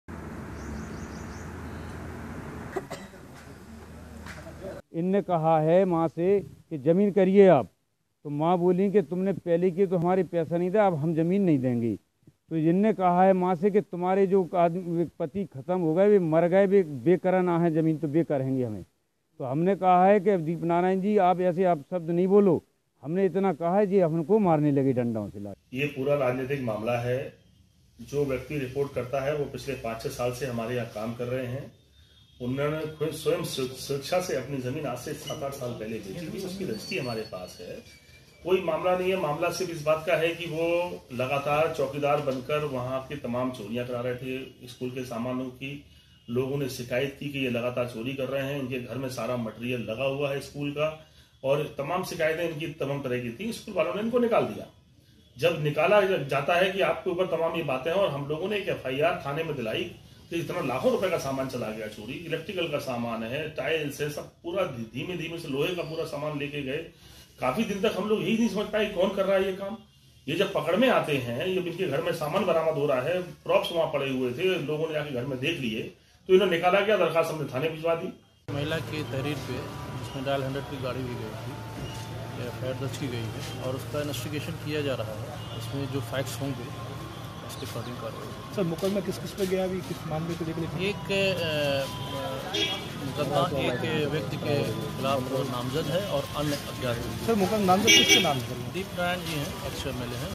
झांसी : पूर्व सपा विधायक पर केस दर्ज, जमीन की जबरन रजिस्ट्री कराने का आरोप
झांसी : समाजवादी पार्टी के पूर्व विधायक दीप नारायण सिंह यादव के खिलाफ जमीन की जबरन रजिस्ट्री कराने के लिए मारपीट करने के आरोप में केस दर्ज किया गया है. पूर्व सपा विधायक पर महिला से बदसलूकी का भी आरोप लगा है.